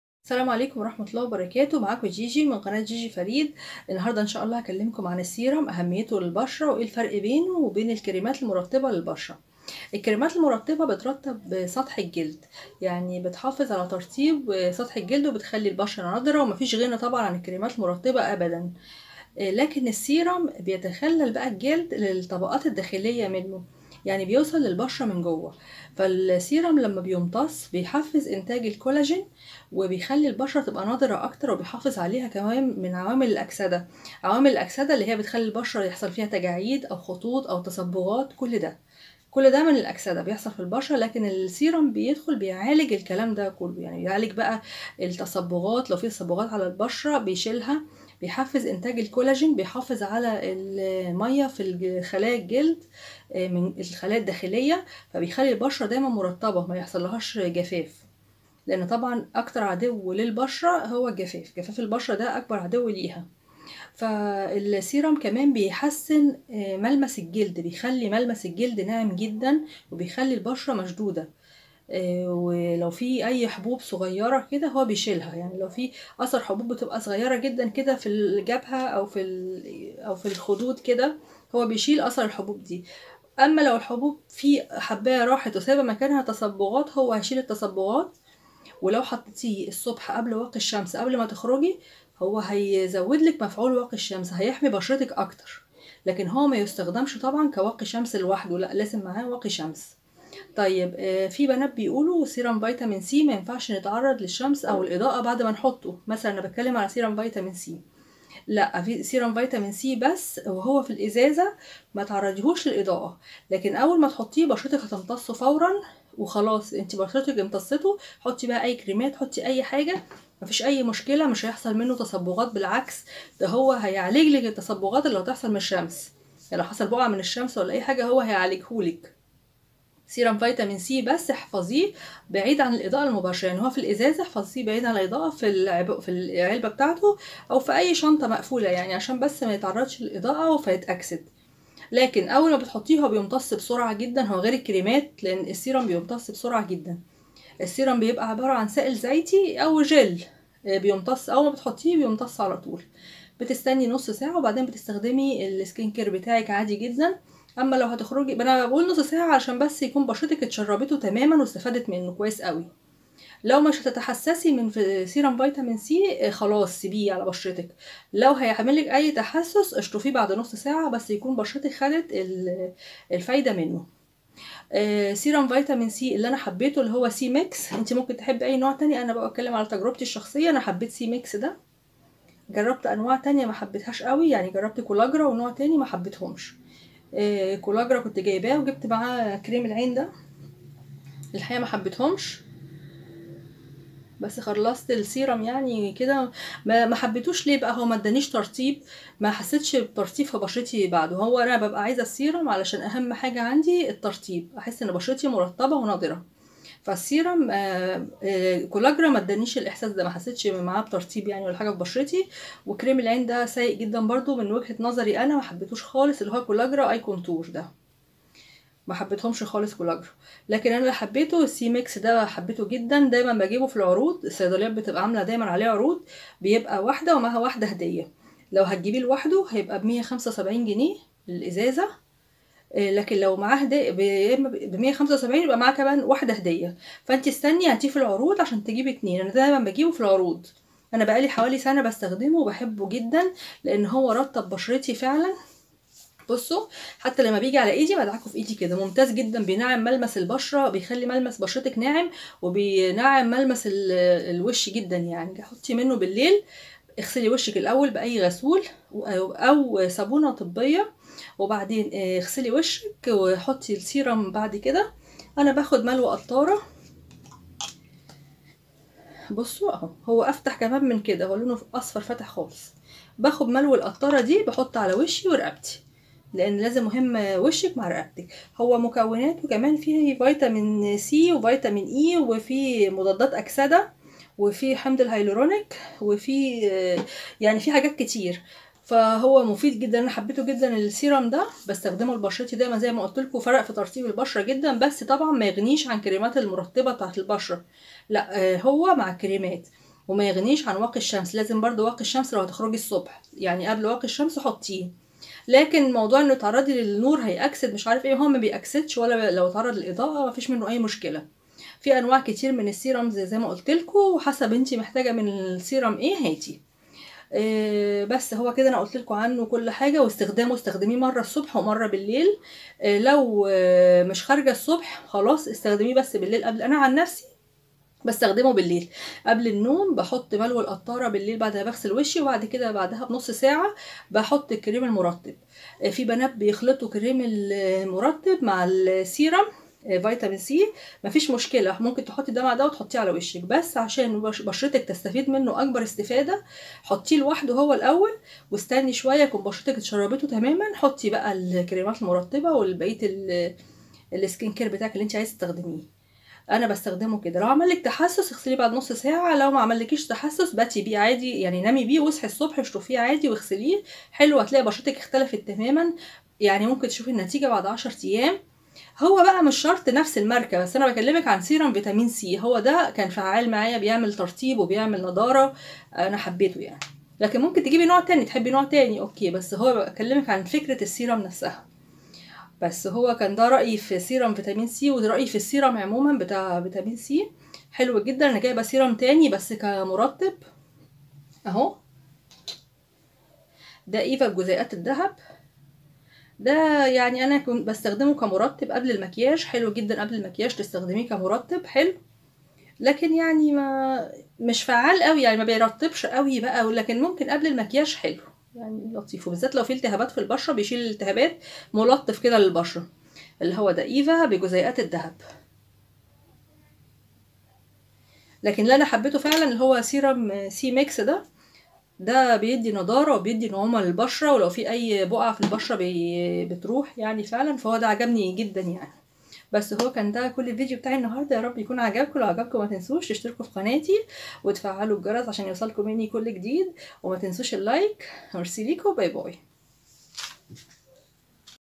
ريفيو سيرم فيتامين C لنضارة وتفتيح البشرة وانواع السيرم اللي جربتها ورايي فيهم
الفرق بين الكريم المرطب للبشرة والسيرم  وانواع السيرم اللي  جربتها واكتر سيرم حبيته وفرق  مع بشرتي وطريقة استخدام السيرم